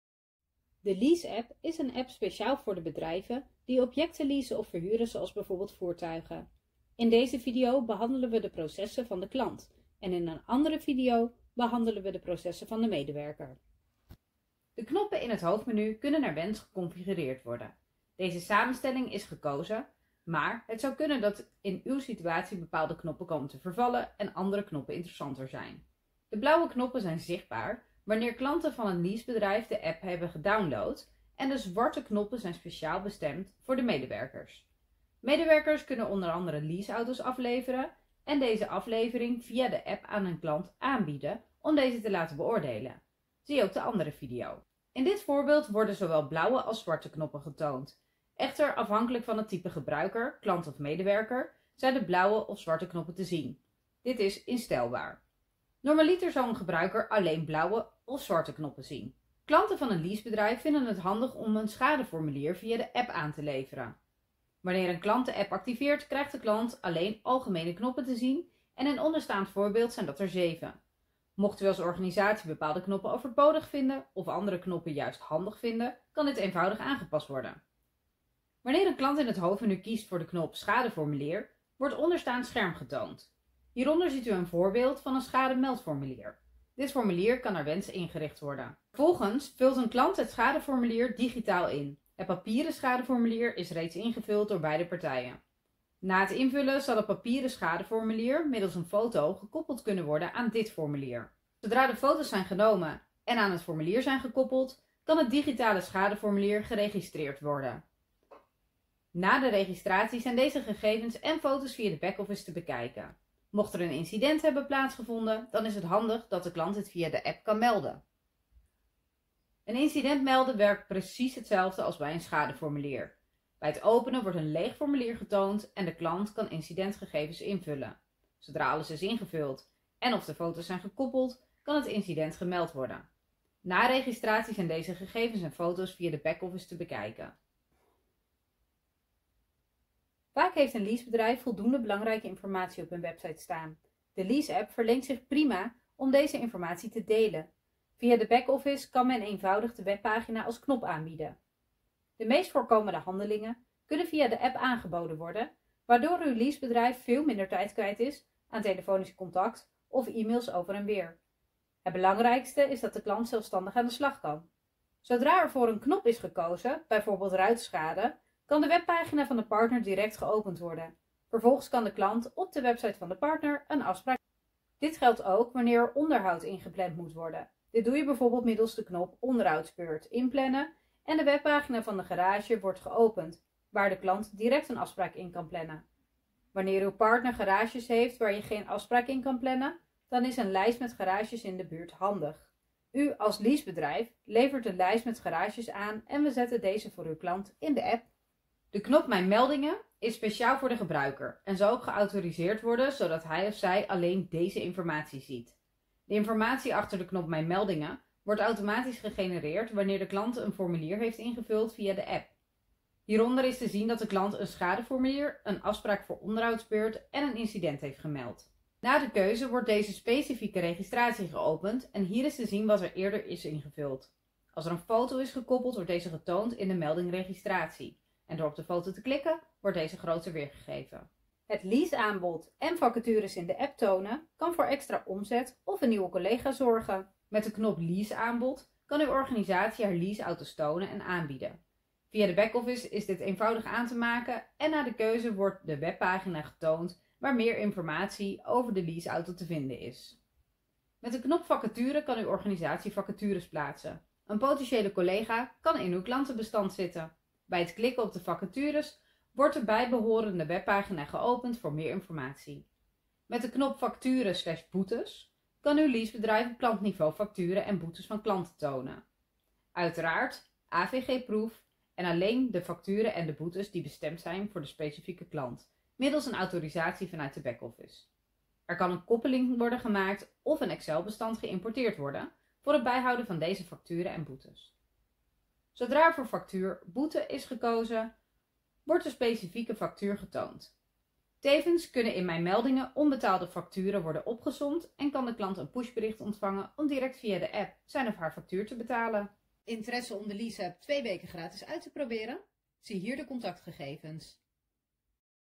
Lease-app: Klant
De lease-app is een app speciaal voor de bedrijven die objecten leasen of verhuren zoals bijvoorbeeld voertuigen. In deze video behandelen we de processen van de Klant (blauwe knoppen) en in een andere video behandelen we de processen van een medewerker (zwarte knoppen). De knoppen in het hoofdmenu kunnen naar wens geconfigureerd worden.